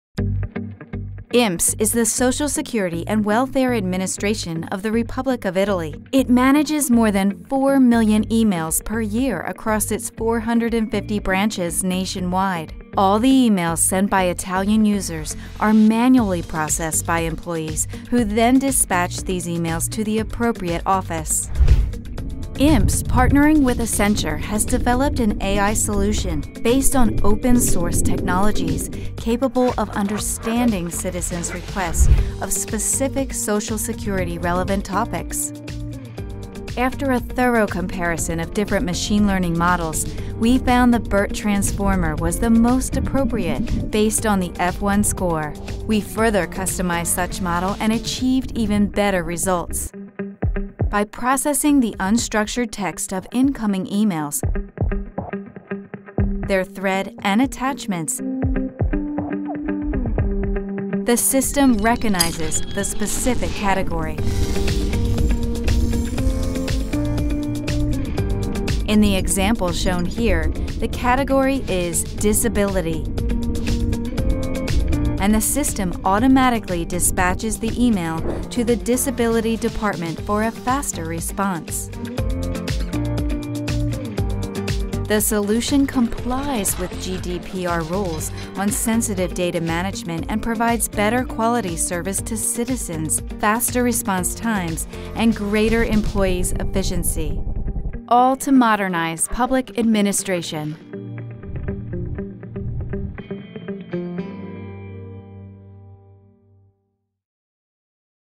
Progetto INPS classificazione e smistamento automatico delle PEC (english version)
Il video in lingua inglese realizzato da Inps che illustra il progetto di classificazione e smistamento automatico delle PEC inserito nella Top 10 mondiale dell'Intelligenza Artificiale da IRCAI, International Research Centre on Artifical Intelligence, di UNESCO